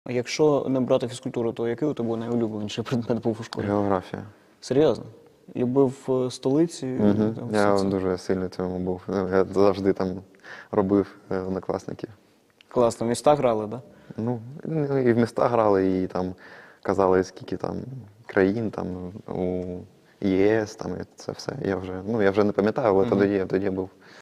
В ЯКОМУ ШКІЛЬНОМУ ПРЕДМЕТІ БРАЖКО ТОП❓️
Володимир Бражко з Динамо Київ у школі дуже любив географію